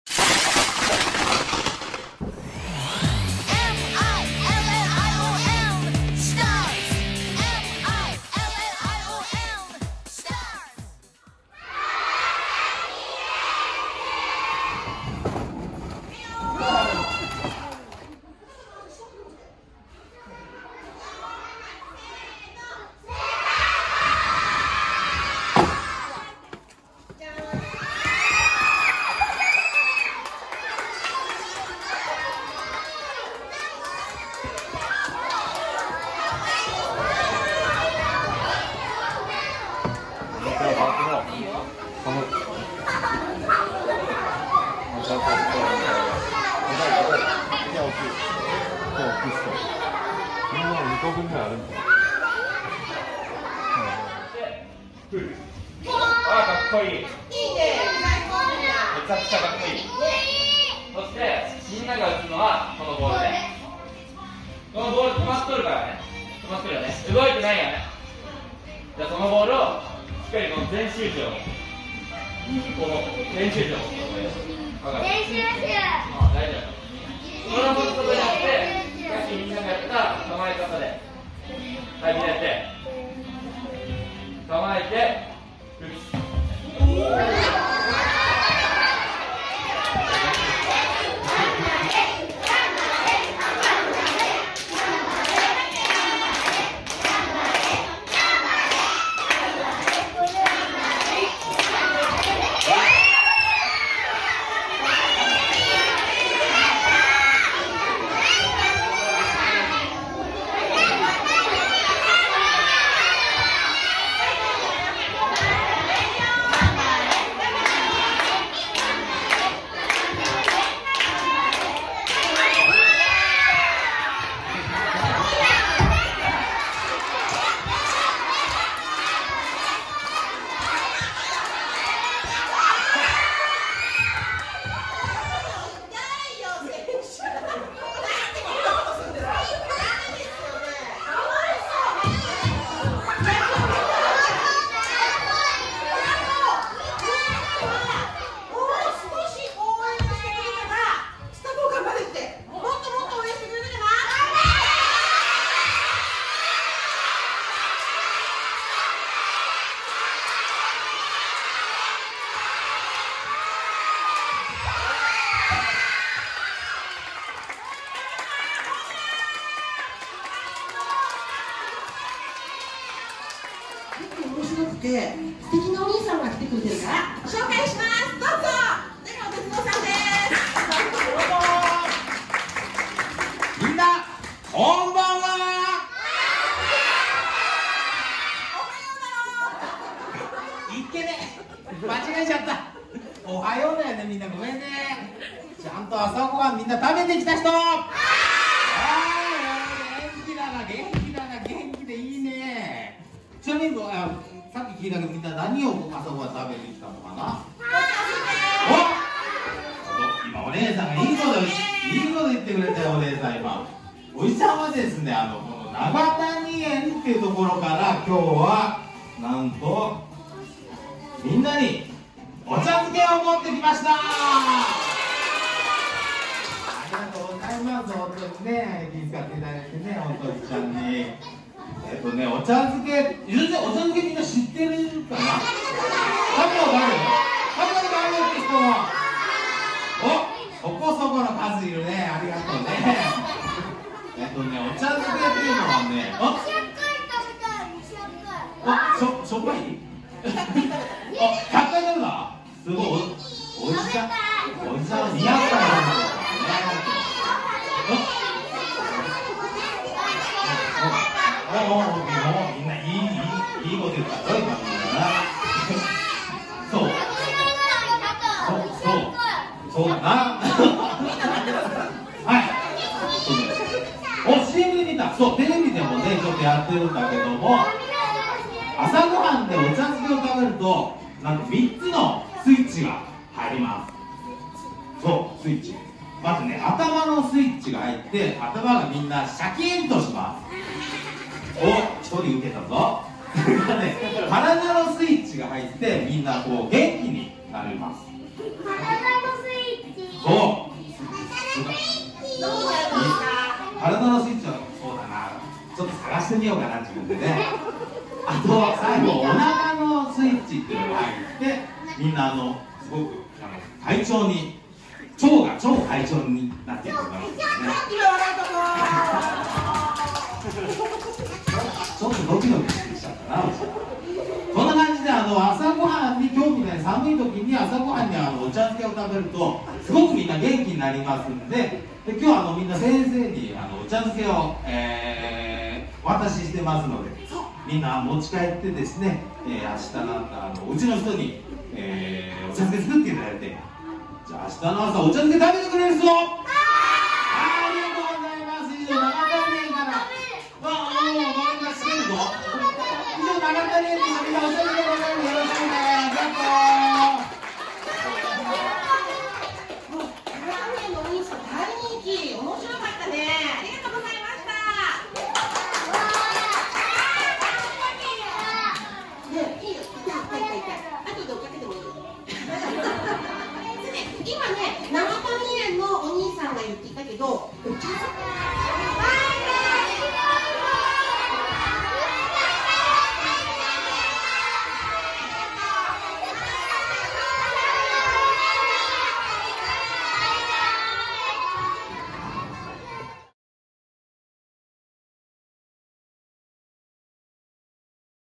スタ坊&川﨑俊哲選手が園児とふれあい‼︎【石川ミリオンスターズ2023】
プロ野球独立リーグ　日本海リーグ
石川ミリオンスターズ2023年2月21日(火)
「"永谷園プレゼンツ"スタ坊キャラバン@かみやちこども園」